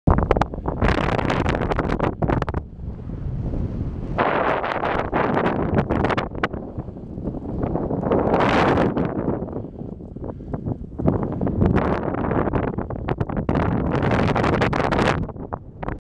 Above Cavedale, Castleton, Derbyshire
Great conditions above Cave Dale. you can see why and how people get lost